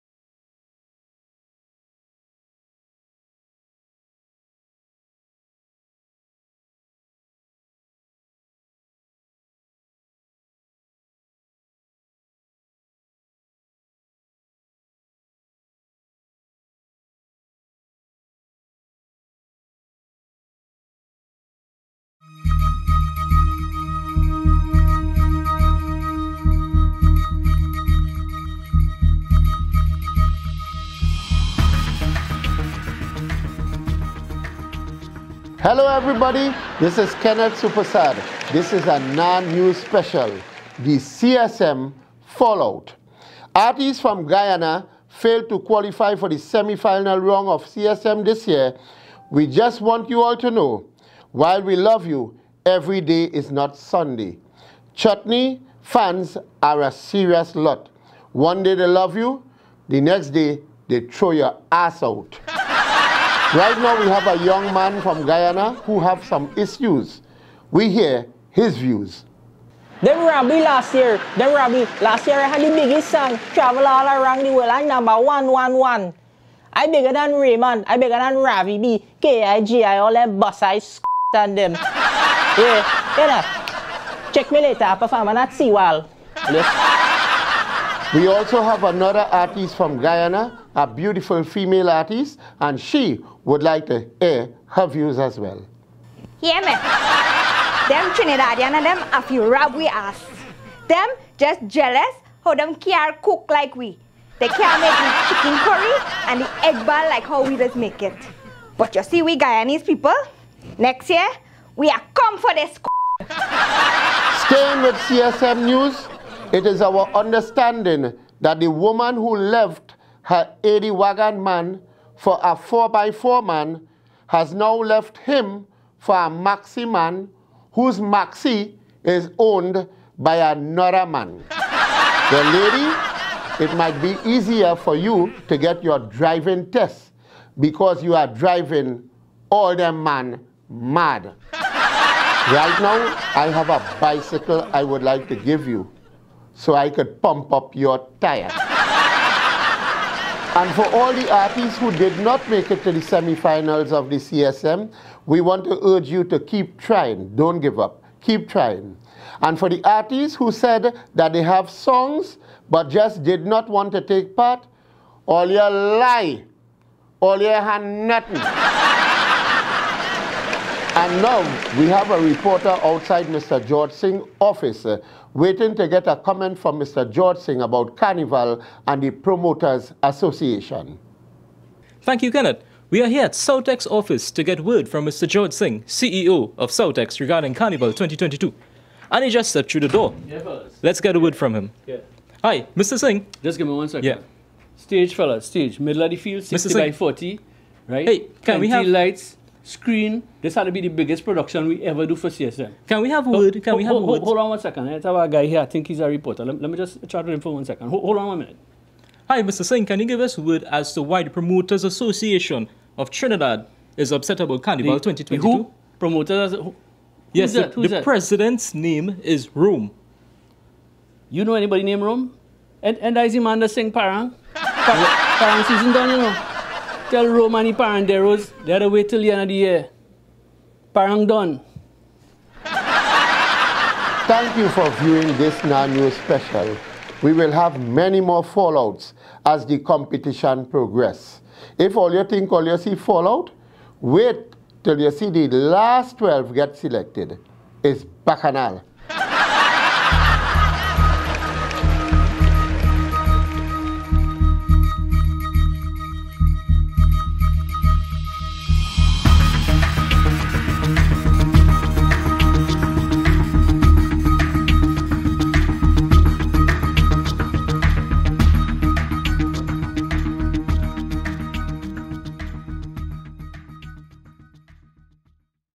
Carnival Bacchanal - A Nah News Special!
This is a Nah News Special! Carnival is Bacchanal! (Remember folks, this is a comedy show! We just drinking water.)